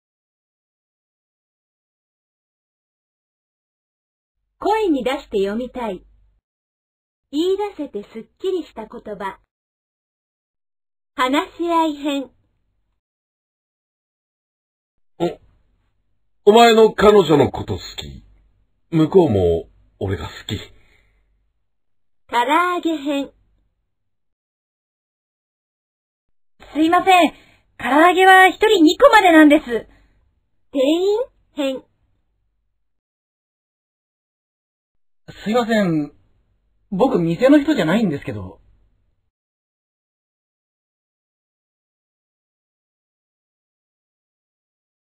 【声に出して読みたい】言い出せてスッキリした言葉 話合い編他
世の中、言いづらいことでイッパイ！
だから、さぁ、テキストを開いて
キッパリ言う練習をしましょう！